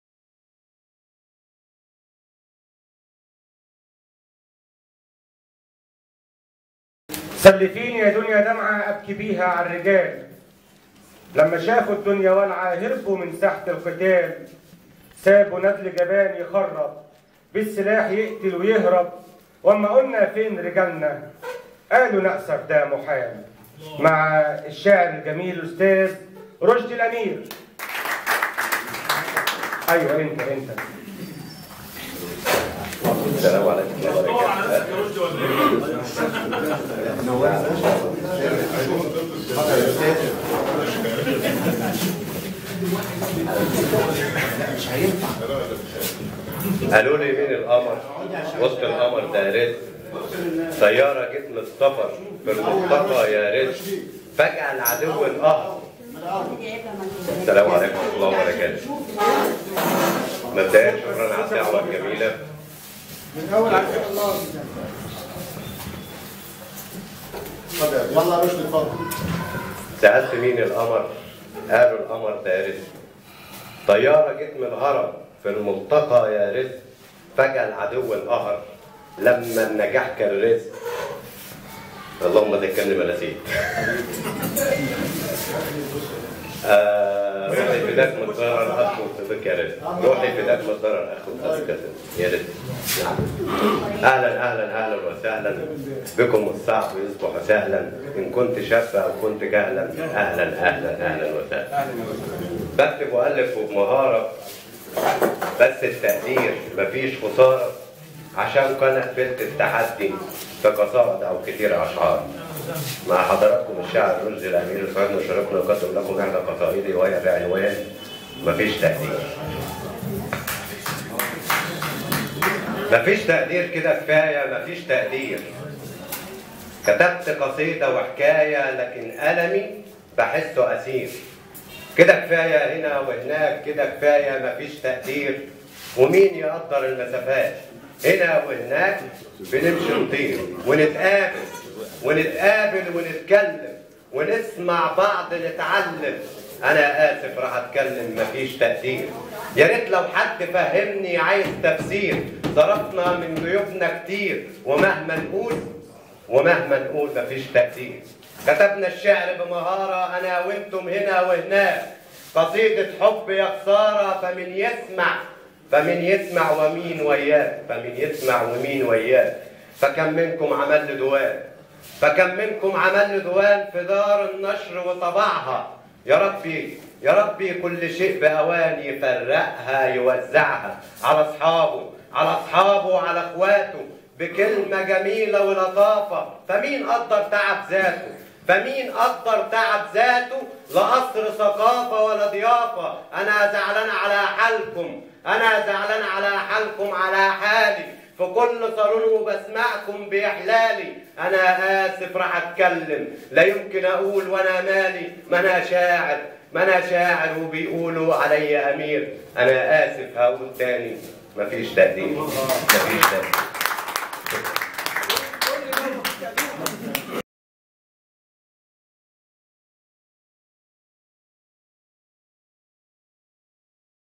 ندوة صالون ملتقى المبدعين للشاعر/ رزق محمد /الشاعر / رشدي الأمير/الخميس 21/12/2023
أمسيةٌ ثقافيةٌ
ندوة صالون ملتقى المبدعين
مؤسس ورئيس الصالون الشاعر الأستاذ / رزق محمد
مشاركة الشاعر الأستاذ / رشدي الأمير
المكان : مؤسسة عبد القادر الحسيني الثقافية